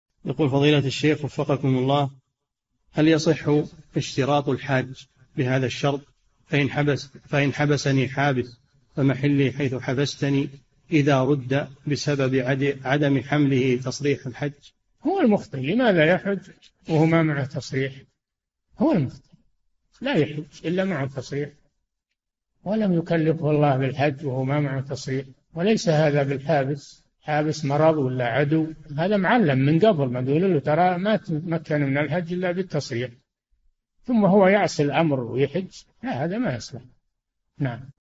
[3489] حكم الحج من غير تصريح - الشيخ صالح الفوزان
"[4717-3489] هل يصح اشتراط الحاج بهذا الشرط: فإن حبسني حابس فمحلي حيث حبستني إذا رُدت لسبب عدم حمله تصريح الحج؟
الجَوَابُ: هو المخطئ لماذا يحج وهو ما معه تصريح؟ هو المخطئ لا يحج إلا مع التصريح ولم يكلفه الله بالحج وهو ما معه تصريح وليس هذا بالحابس، الحابس مرض أو عدو، هذا أُعلم من قبل أنه لن يتمكن من الحج إلا بالتصريح ثم هو يعصي الأمر ويحج، هذا ما يصلح!

للاشتراك في قناة الشيخ صالح الفوزان

للاستفسار والإقتراحات والدعم
رئيس الفريق
لمشروع كبار العلماء